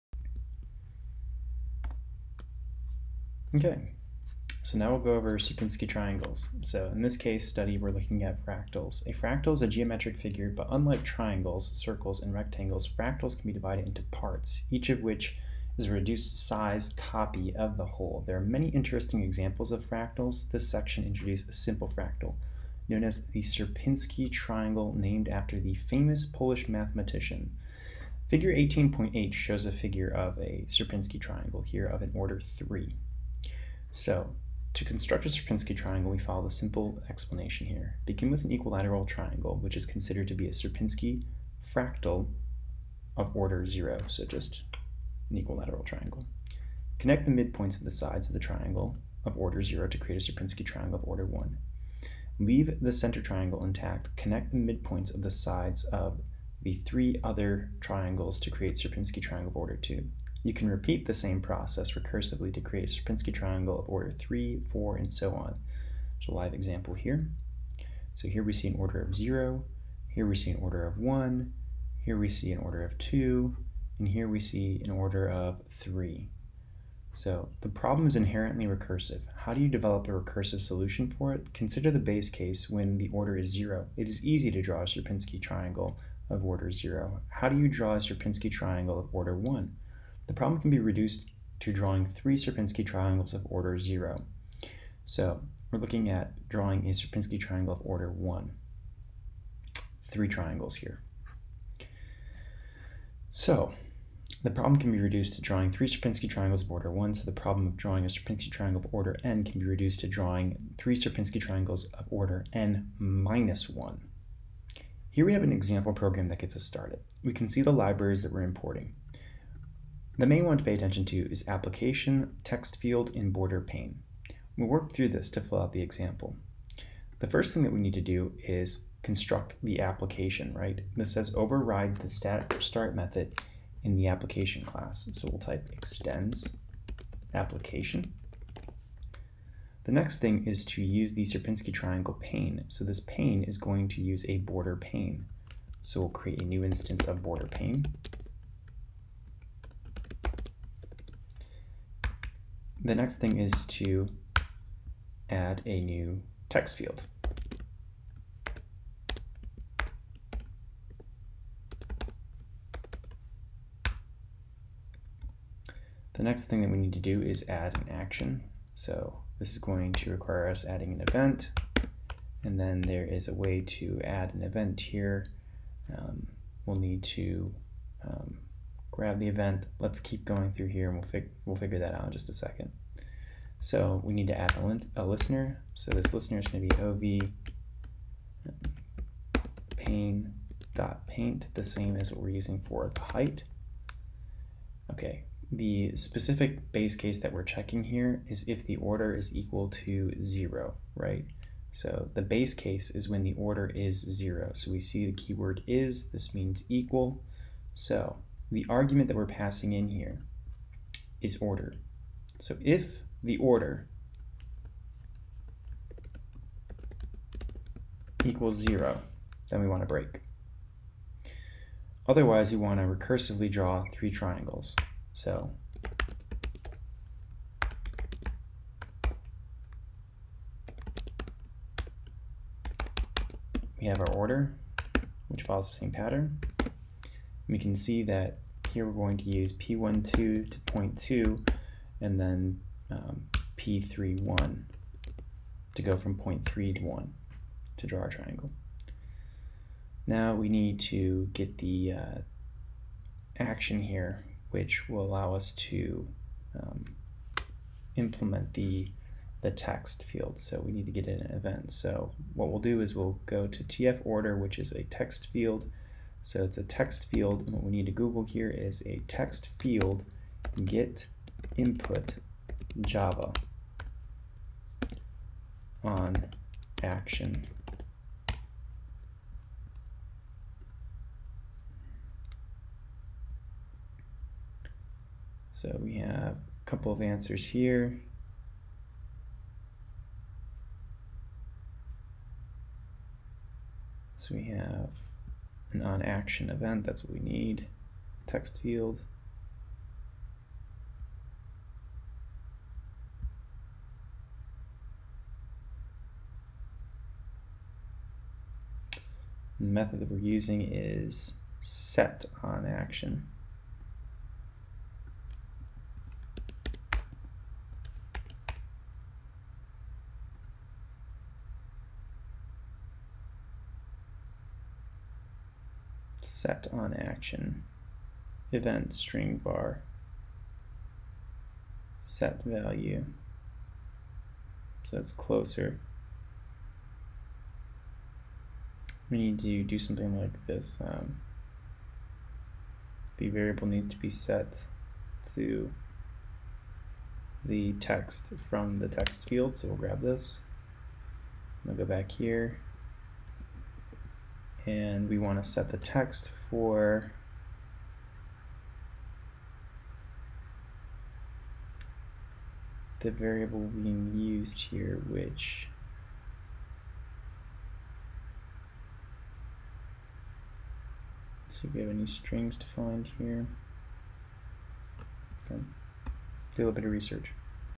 Sierpinski triangle in Java
How to make a Sierpinski triangle in Java part 1